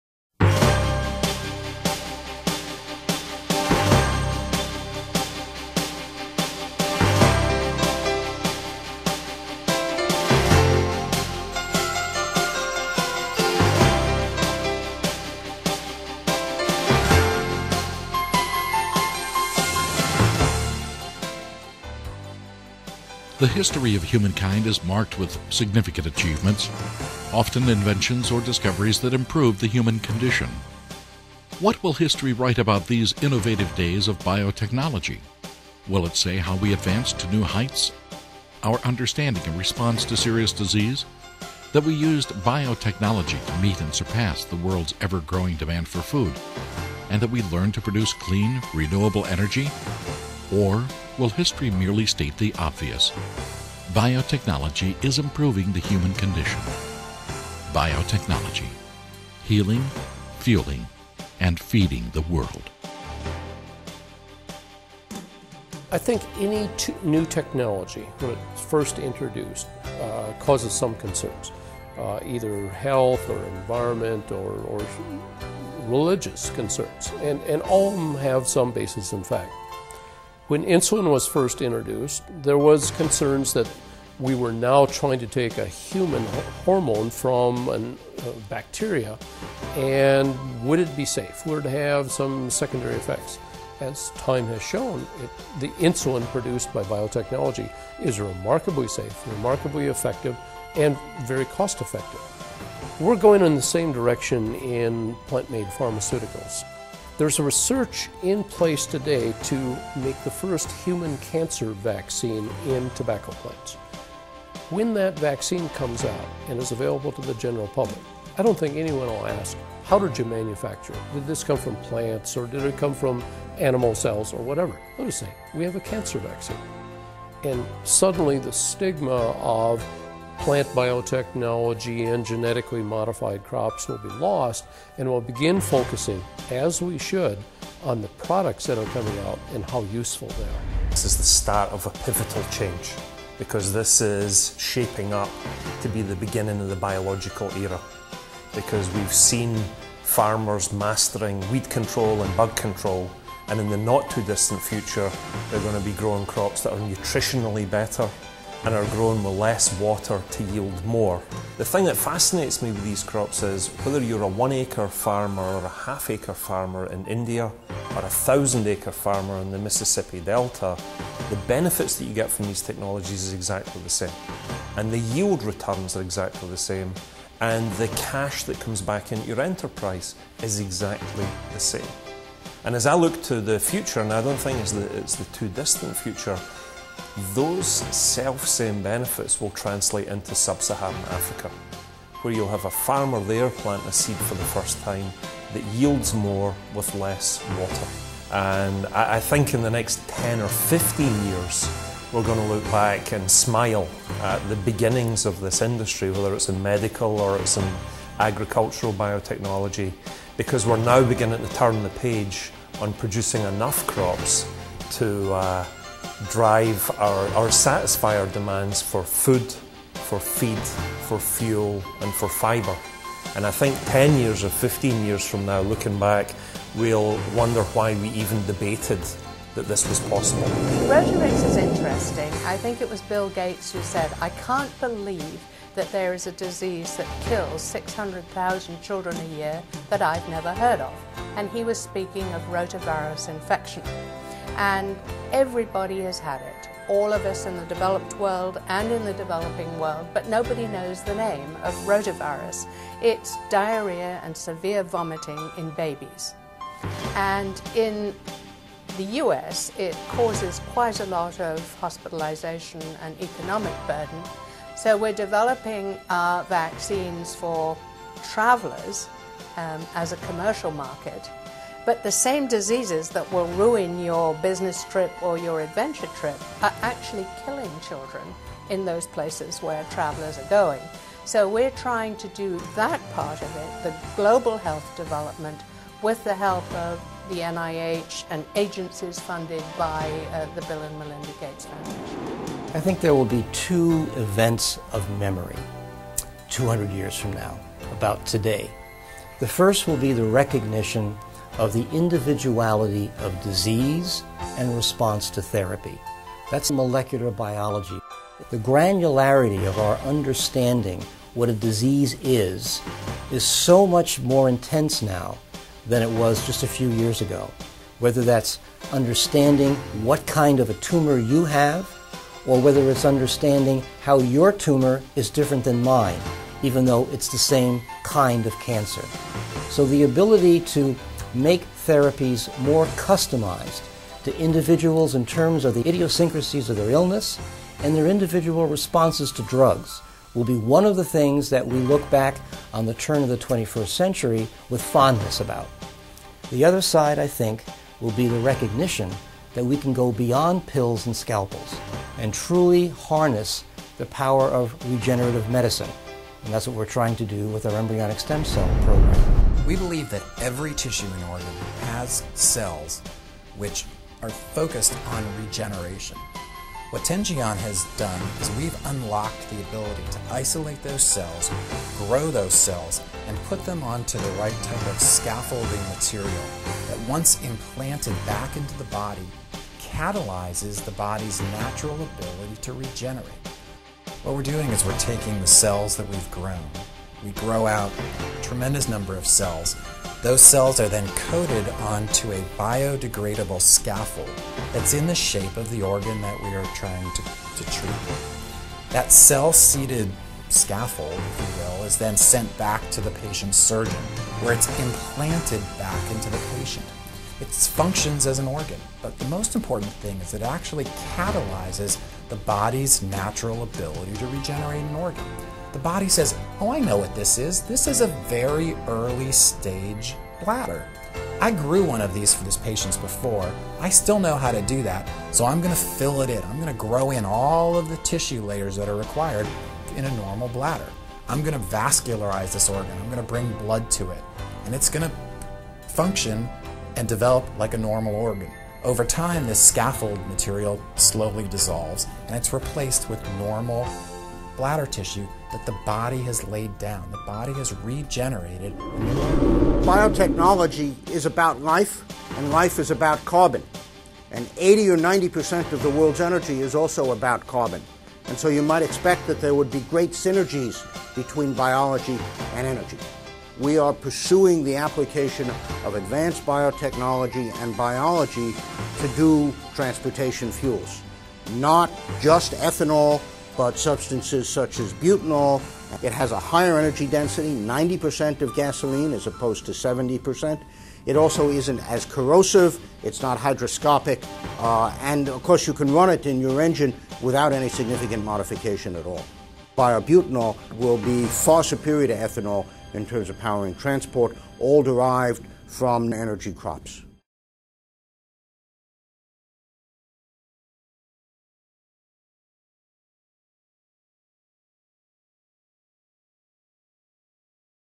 The Age of Biotechnology
The history of humankind is marked with significant achievements, often invention or discoveries that improve the human condition.  What will history write about these innovative days of biotechnology?  Will it say how we advanced to new heights?  Our understanding and response to serious disease?  That we used biotechnology to meet and surpass the world's ever-growing demand for food?  And that we learned to produce clean, renewable energy?  Will history merely state the obvious: Biotechnology is improving the human condition?